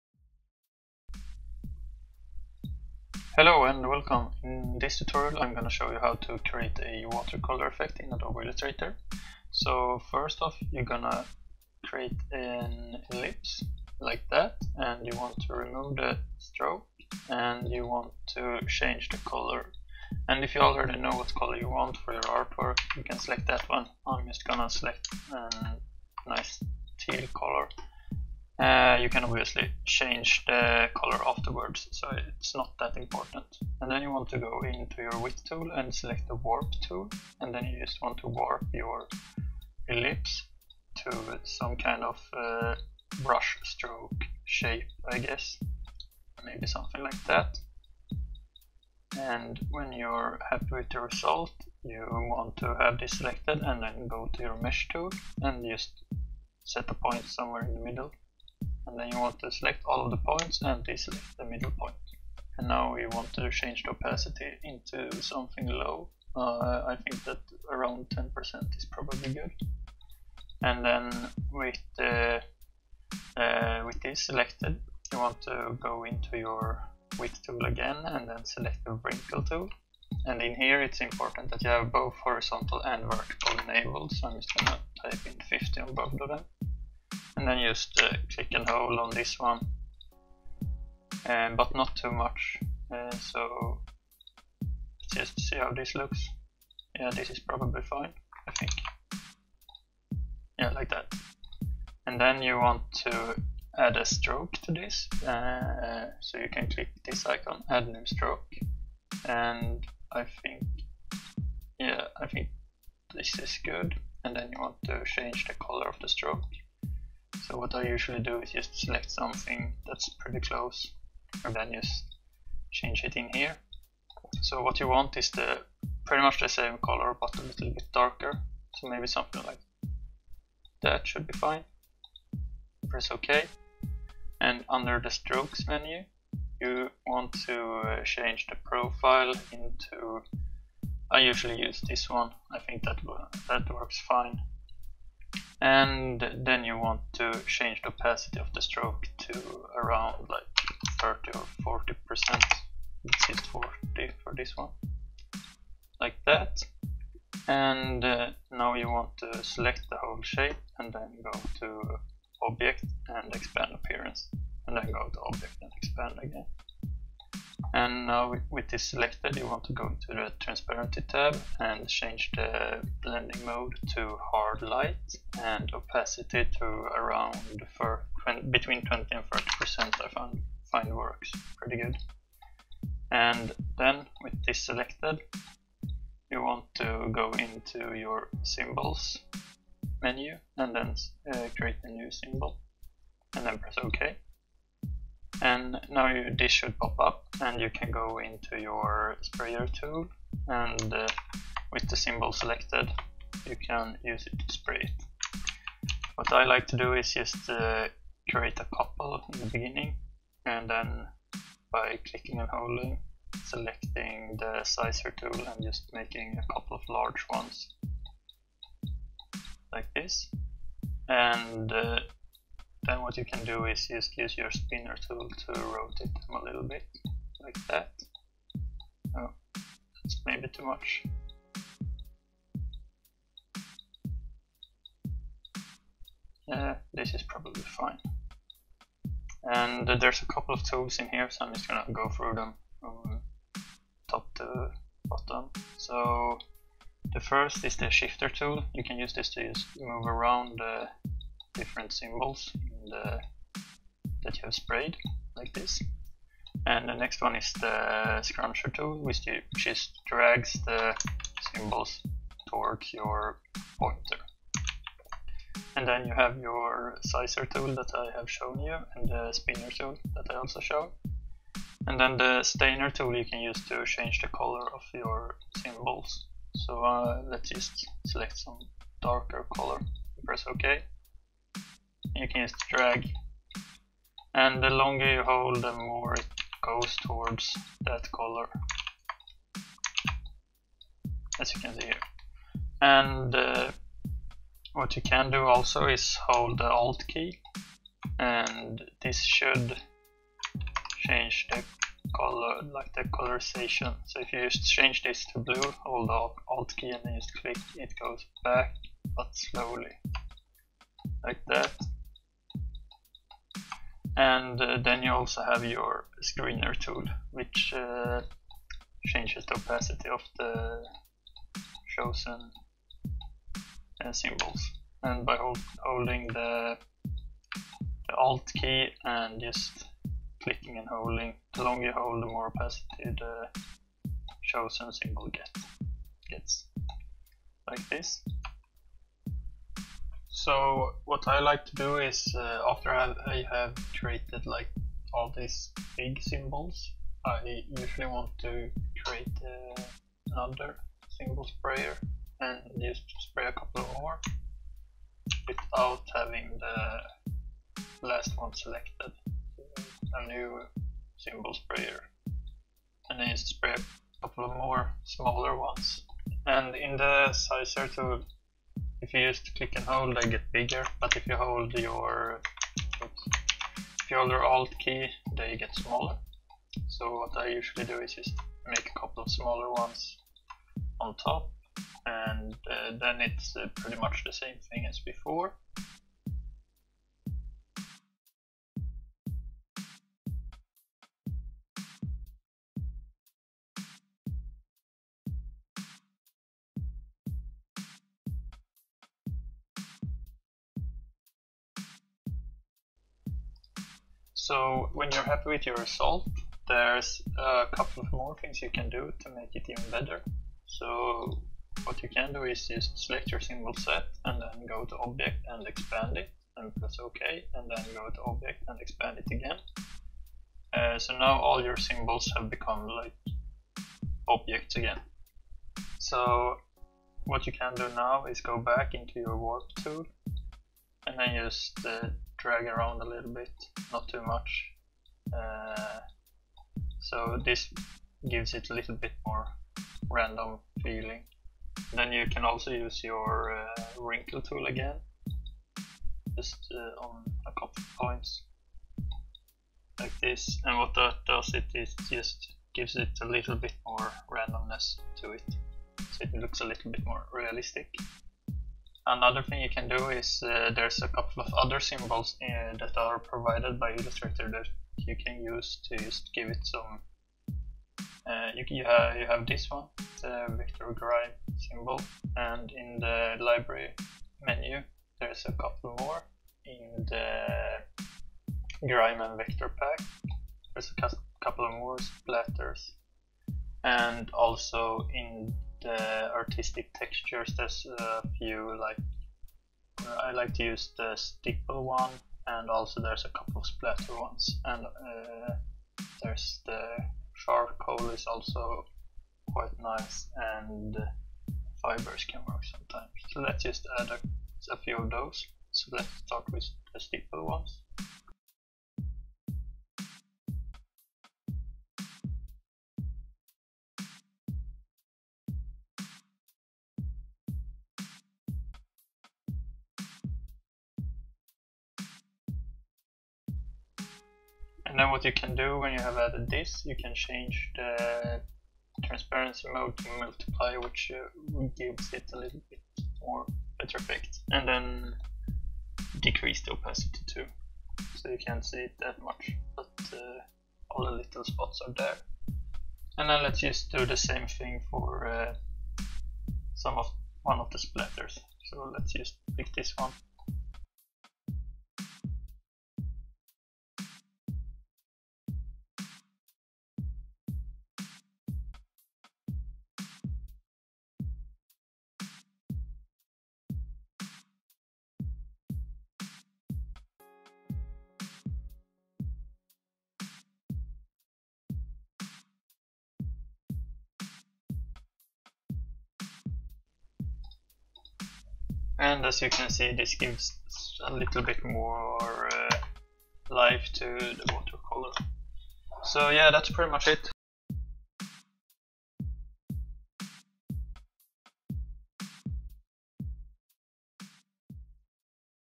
Adobe Illustrator Tutorial Watercolor Effect
This is a tutorial on another way of creating a watercolor effect in adobe illustrator using the symbol sprayer tool.